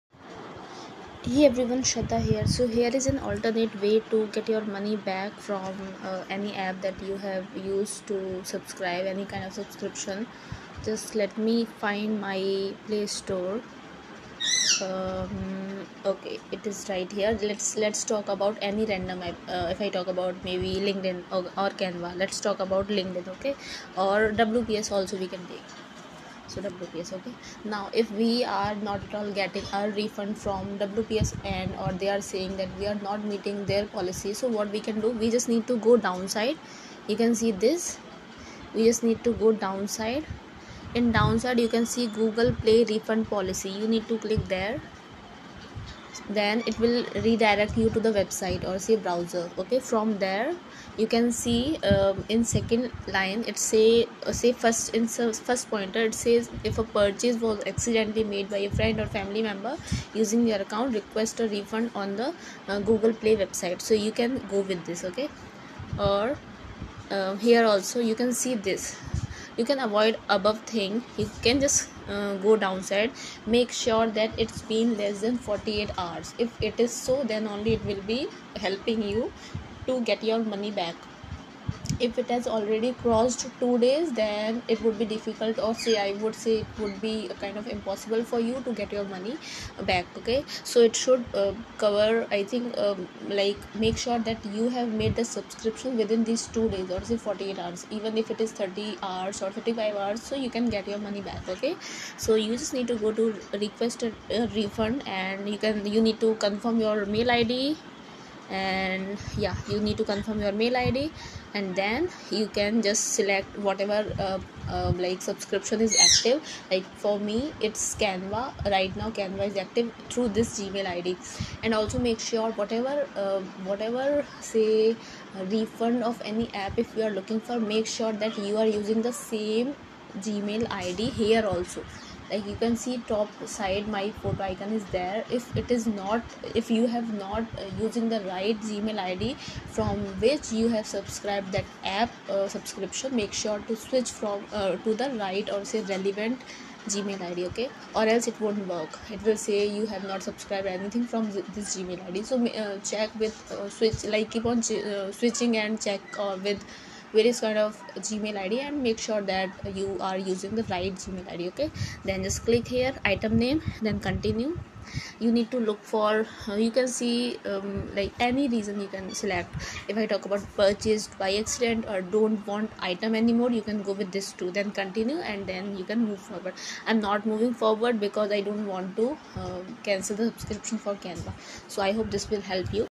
How to refund your money from WPS/LinkedIn/Canva/Any other app|Alternate way🔥
Here's a step-by-step guide on how to unsubscribe or request a refund from popular apps on Windows:

*WPS Office:*

1. Open WPS Office
2. Click on the "Me" icon (top right corner)
3. Select "Account"
4. Click on "Subscription"
5. Choose "Cancel Subscription"

*Canva:*

1. Log in to Canva
2. Click on your profile icon (top right corner)
3. Select "Settings"
4. Scroll down to "Subscription"
5. Click on "Cancel Subscription"

1. Log in to LinkedIn
2. Click on your profile icon (top right corner)
3. Select "Settings & Account"
4. Click on "Account"
5. Scroll down to "Subscription"
6. Click on "Cancel Subscription"

*General steps for other apps:*

1. Open the app
2. Look for the "Settings" or "Account" section
3. Search for "Subscription" or "Membership"
4. Click on "Cancel Subscription" or "Unsubscribe"
5. Confirm cancellation

*Refund requests:*

1. Check the app's refund policy
2. Contact the app's customer support
3. Explain your reason for requesting a refund
4. Provide your subscription details

*Windows Store:*

1. Open the Microsoft Store
2. Click on your profile icon (top right corner)
3. Select "Order history"
4. Find the app subscription
5. Click on "Request a refund"

Remember to review the app's refund policy and terms before requesting a refund.